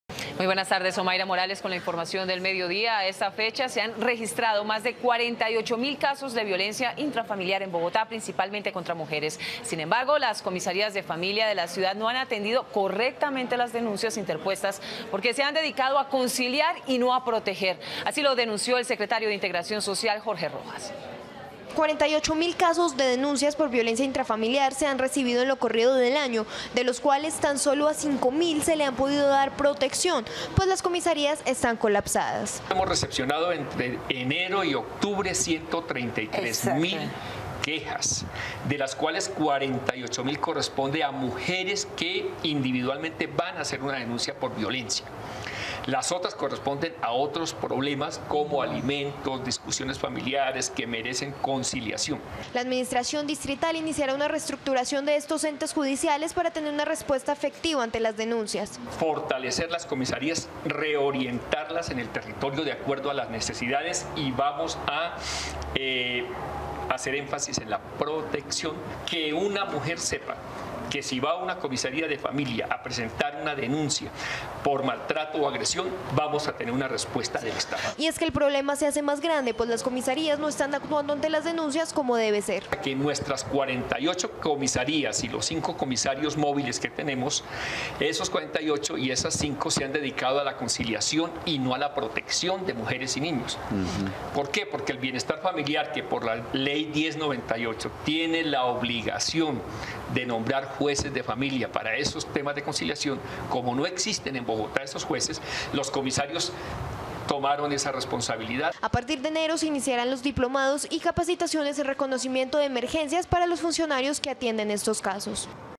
"Comisarías de Familia se han dedicado a conciliar y no a proteger": Sec. Integración
Noticias Capital 12 m. 14 noviembre 2013

A esta fecha se han  registrado más de  48 mil casos de violencia intrafamiliar en Bogotá, principalmente contra mujeres.
Sin embargo,  las Comisarías de Familia de la ciudad no han atendido correctamente las denuncias interpuestas porque se han dedicado a conciliar y no a proteger. Así lo denunció el Secretario de Integración Social, Jorge Rojas.